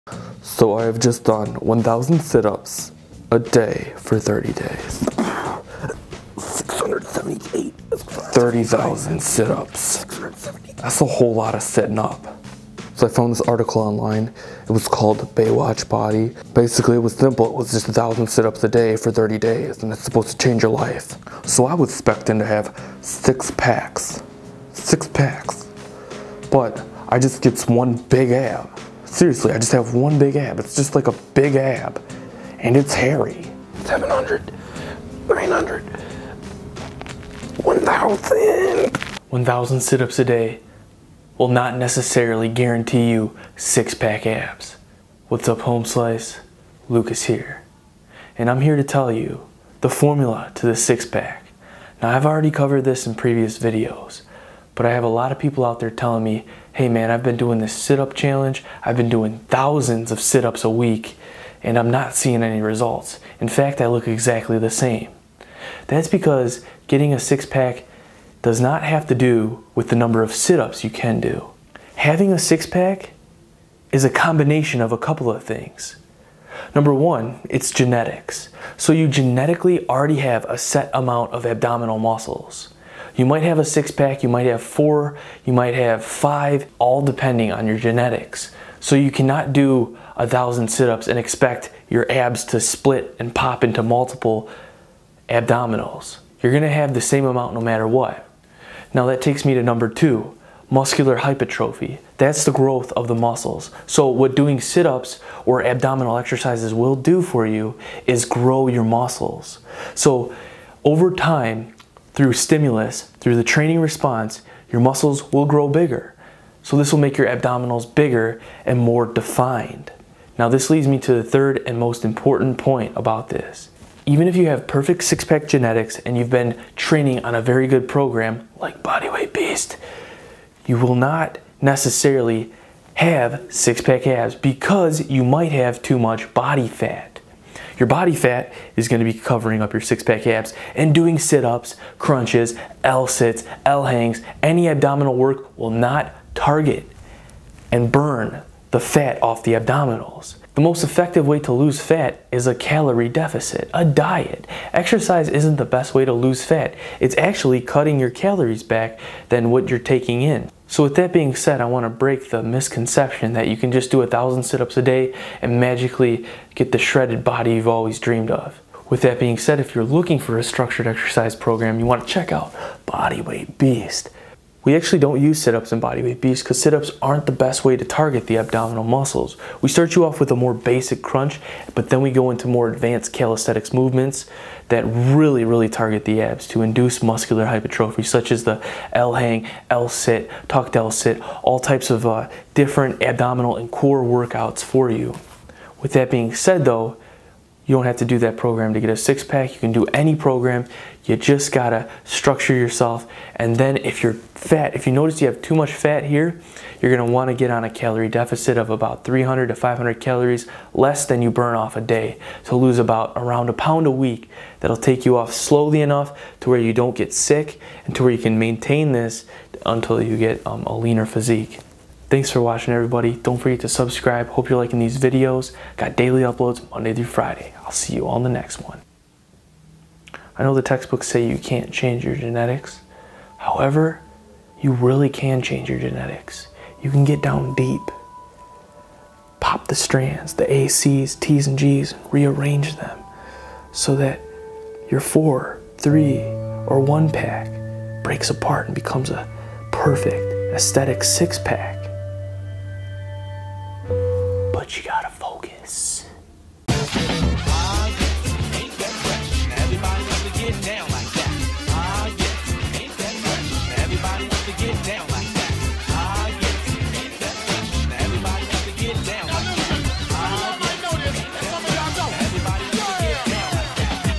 1000 Sit-Ups A Day For 30 Days
Will 1000 situps a day get you 6-pack abs? Will doing sit ups help you get lean? In this video I go over the misconceptions for getting 6-pack abs. What it really comes down to is body fat percentage as well as your genetics. A structured exercise program will help but only with muscular growth. If you would like a structured exercise program then check out Bodyweight Beast!

[Patrons]

Dave B.
Clark R.
Jamie M.
Christopher C.
Lukas B.
Clint H.